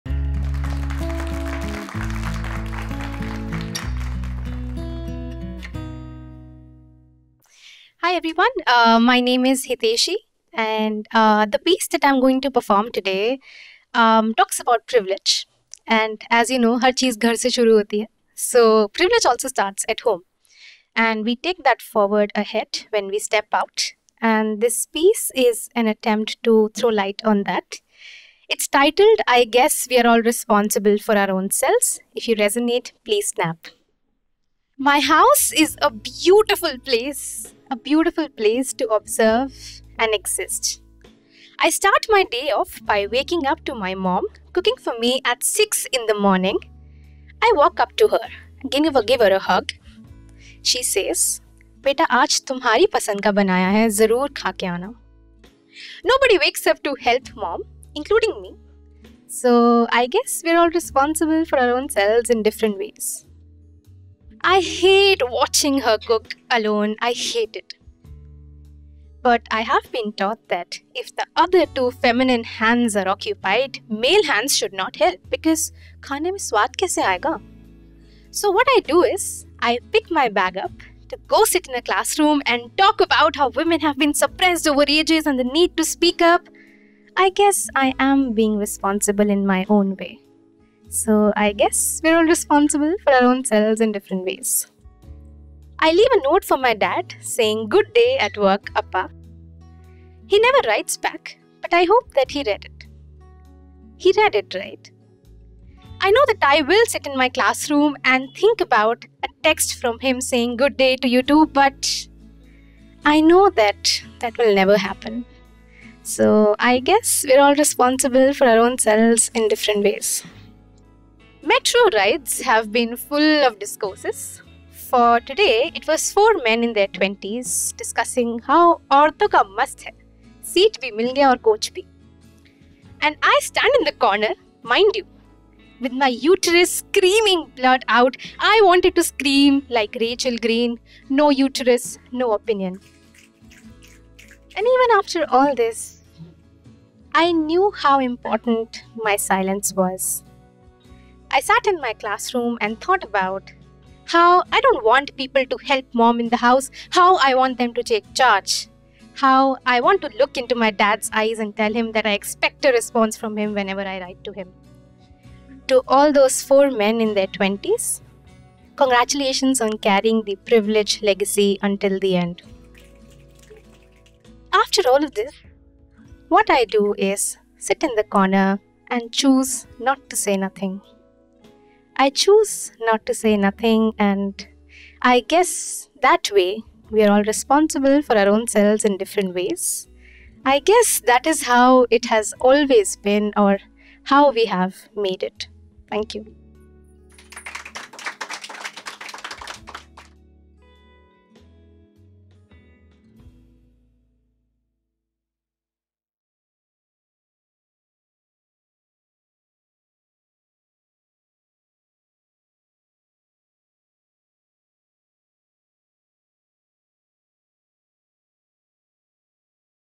We're All Privileged | HauteSlam Ep.4
Aren't we all privileged? If not all, but many of us are. The fact that you're watching this on your phone and are able to comprehend the language, by default puts you in the privileged category. So what are we going to do about it? Or rather can we do anything about it? How can we make a difference?

Hiteshi Gangwani is an aspiring poet and a college student with seering take on the idea of privilege. A riveting poem indeed! You'll love it as much as we did.